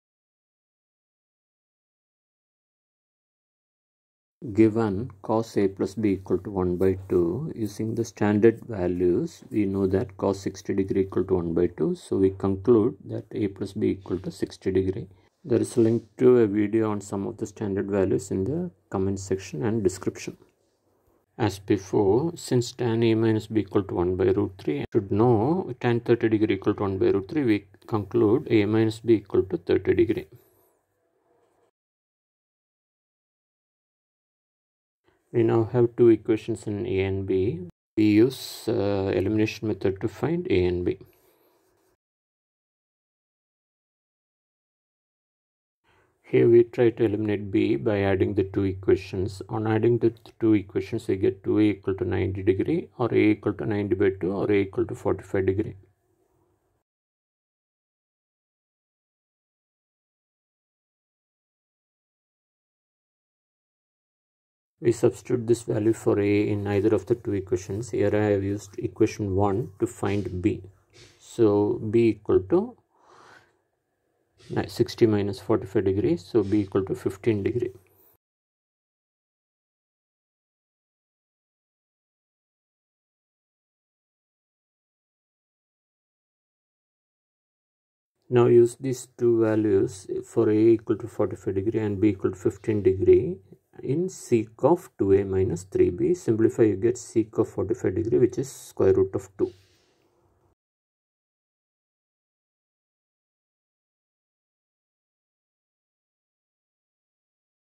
If cos(A+B)=½ , tan(A-B)=1/[√3], find the value of sec(2A-3B), if (A+B) is an acute angle
If cos(A+B)=½ , tan(A-B)=1/[√3], find the value of sec(2A-3B), if (A+B) is an acute angle 0 less than or equal to (A+B) less than or equal to 90°

cbse supplementary exam June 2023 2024 question paper question 25 trigonometry standard ratios